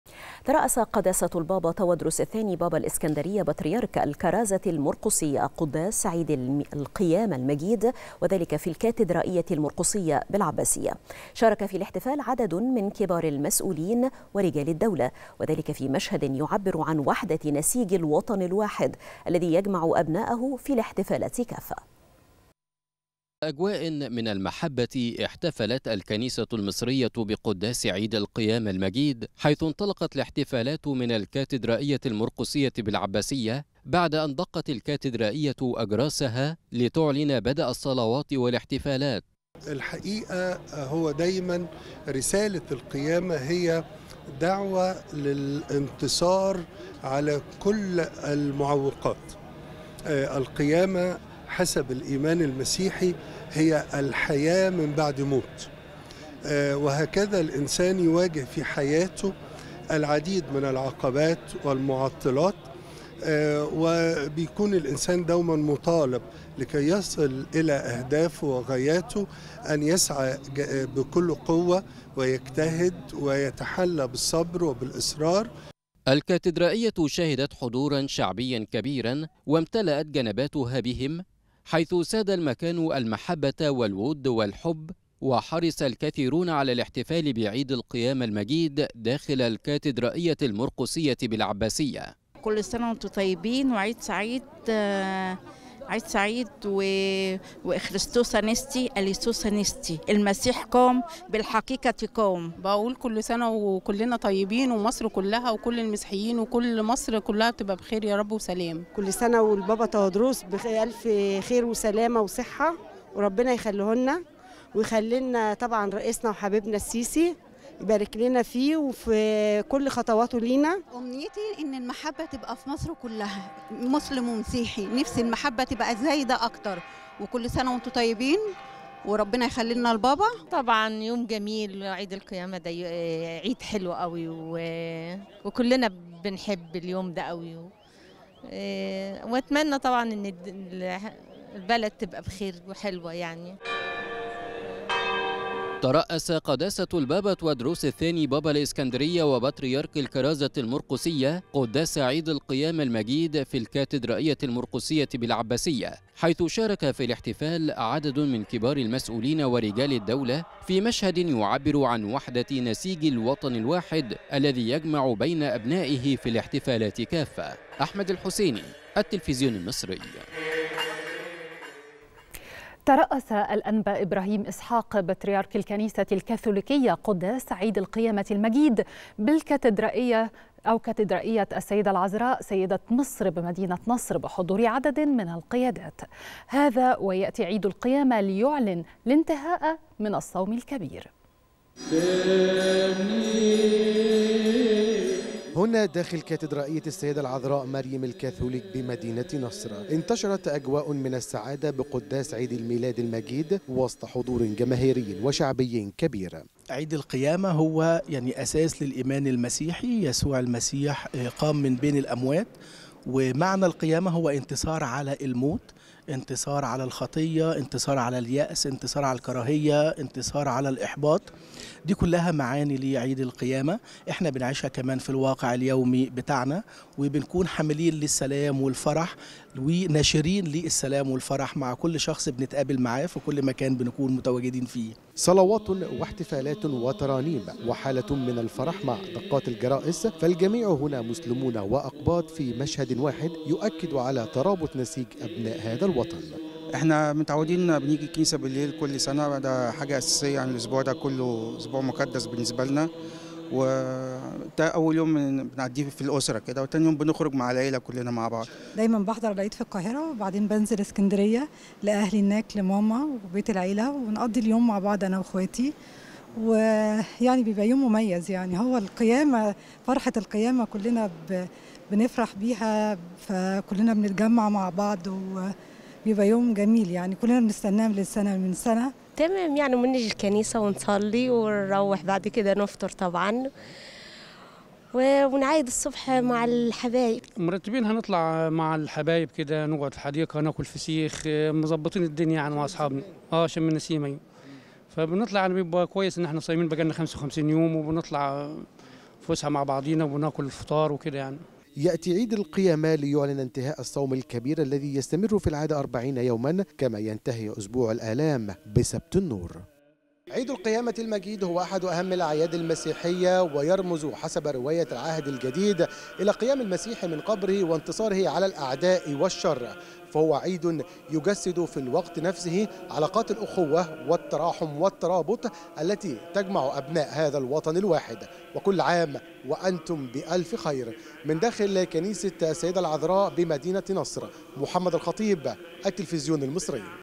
قداسة البابا تواضروس الثاني بابا الإسكندرية يترأس قداس عيد القيامة
نشرة الثالثة | قداسة البابا تواضروس الثاني بابا الإسكندرية يترأس قداس عيد القيامة